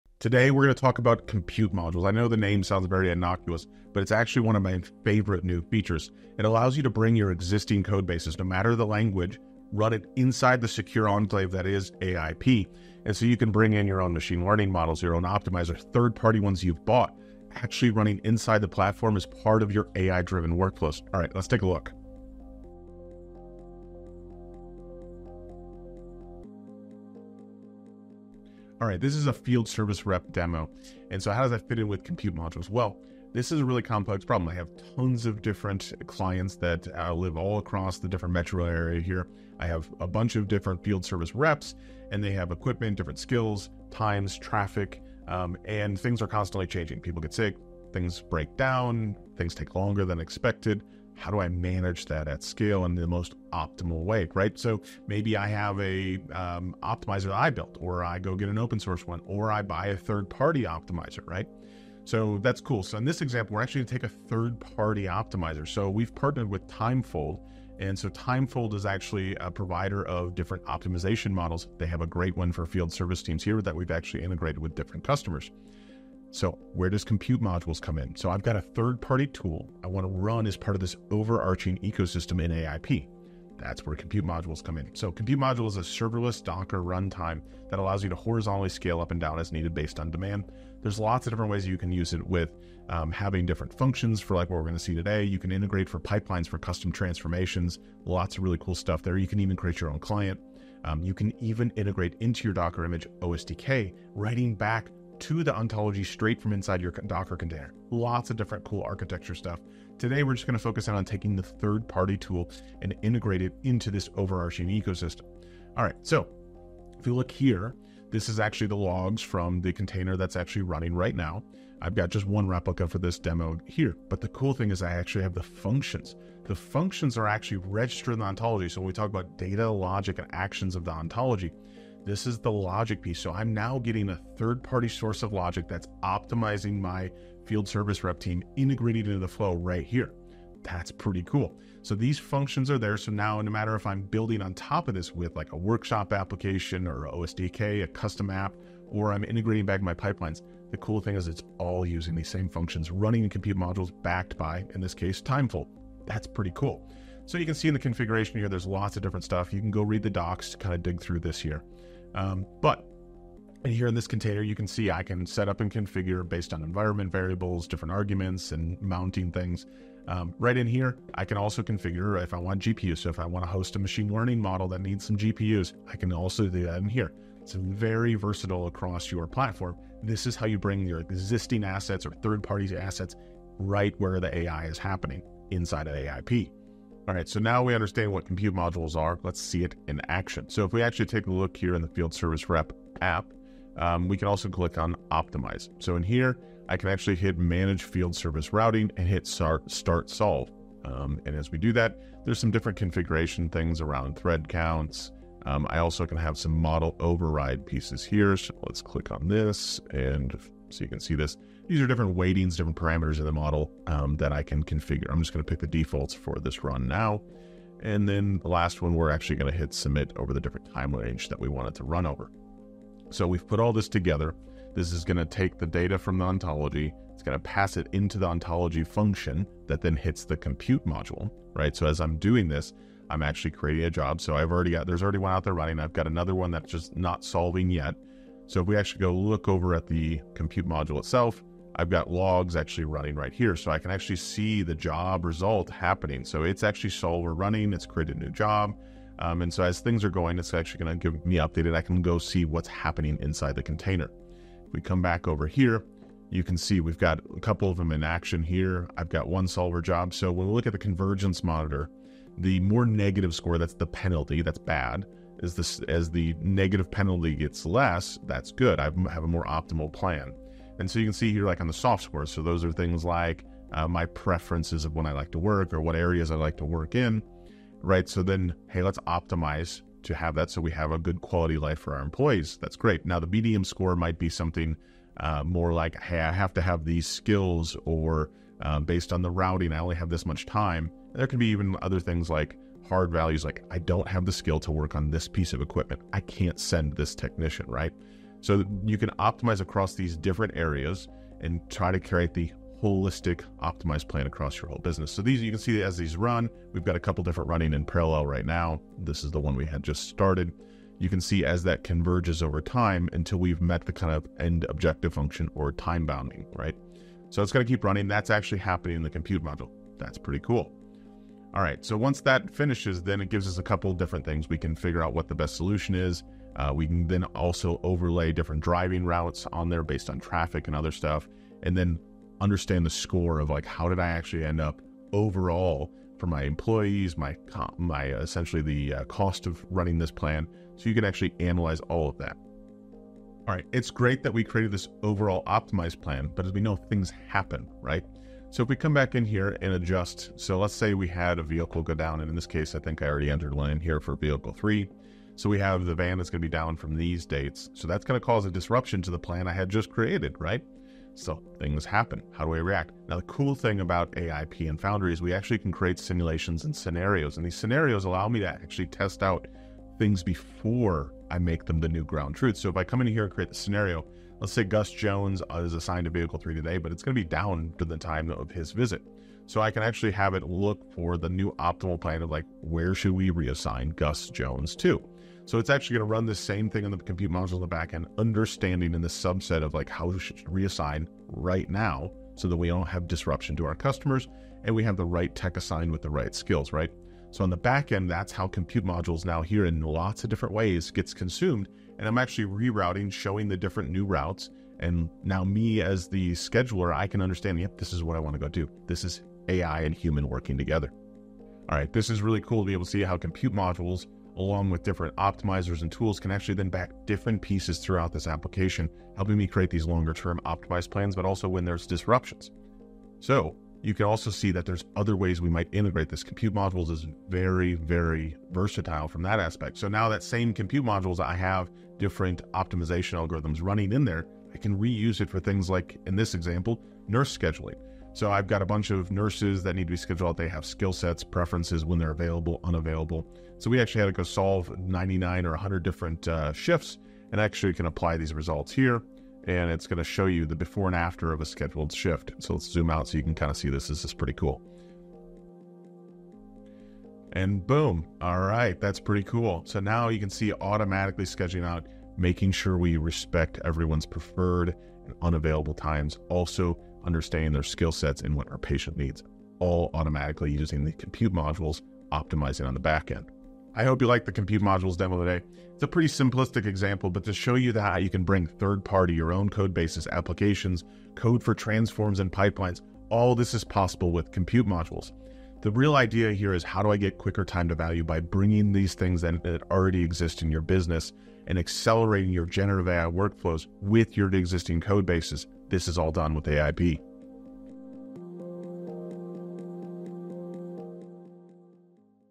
Build with AIP: Compute Modules
Let's take a look at Chad’s favorite new feature, Compute Modules, and see how he is using it for a field service rep team.

While the name may sound innocuous, Compute Modules allows you to bring in your existing code base—regardless of the language—and run it inside AIP.

Whether you are incorporating your own sources of logic (optimizers, ML models, custom transformations, etc.) or third-party ones you've bought, this feature enables you to run them inside the platform as part of your AI-driven workflows.